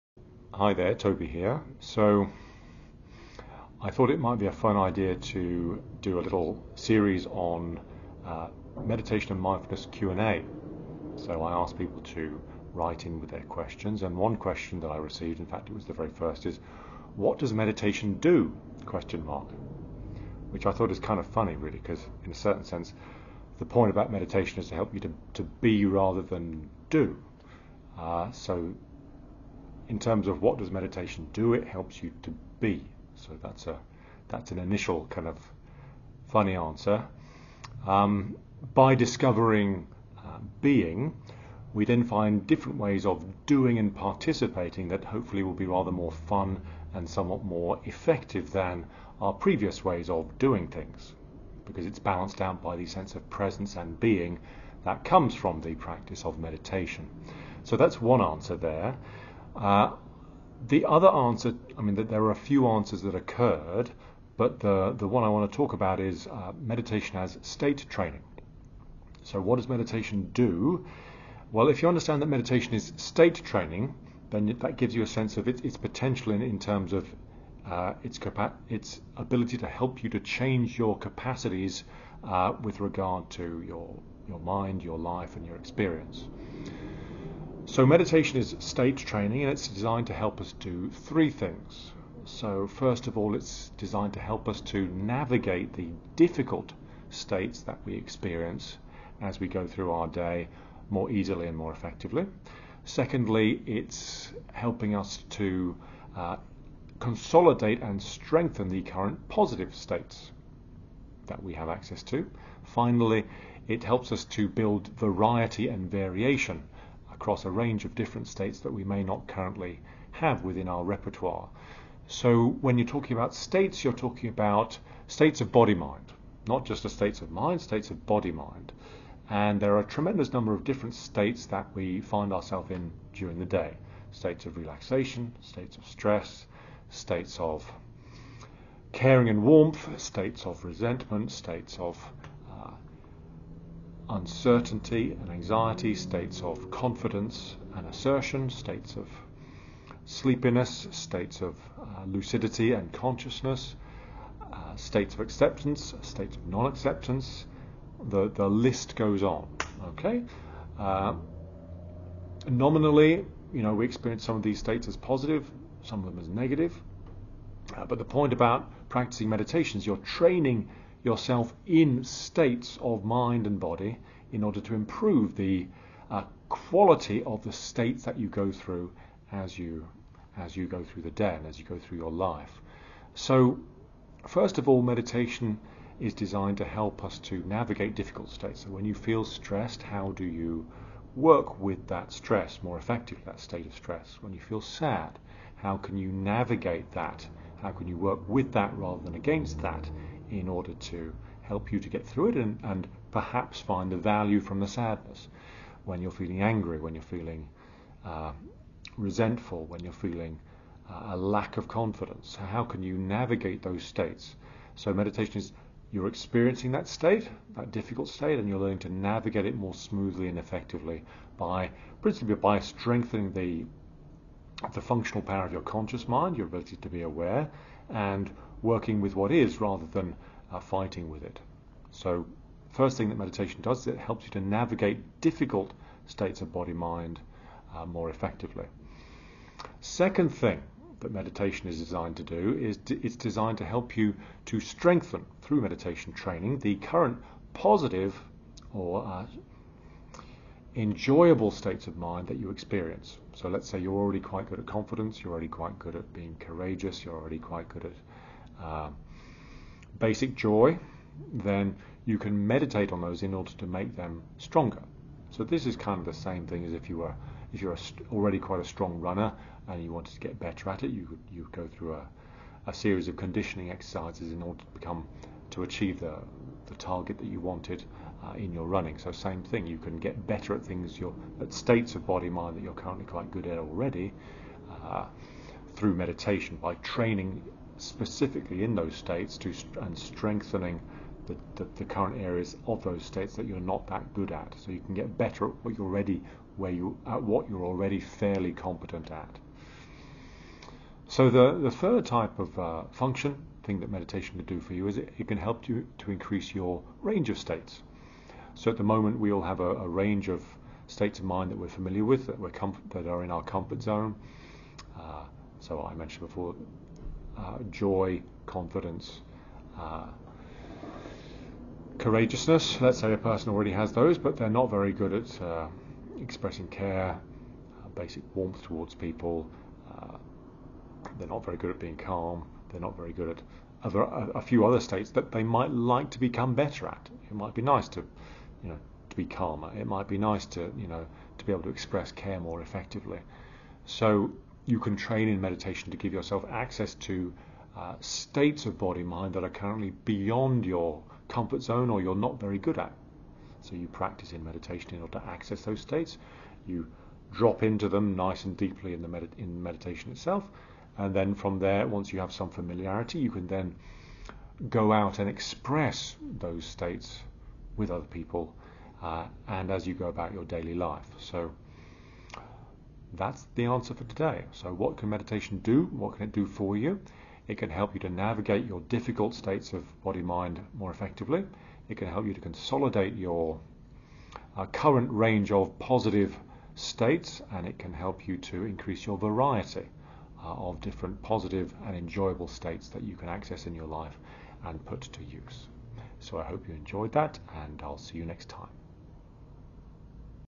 What can meditation do? - Meditation & state training
Part of the 'Questions on the path series' that I am starting, any questions you have on meditation & mindfulness, put them in the comments below!

The main body of my answer relates to state training meditation helps you to:
- Navigate difficult states of body-mind more easily
- Strengthen your current range of positive states
- To expand the range of useful states available to us, like an artist expanding the colour range with which she works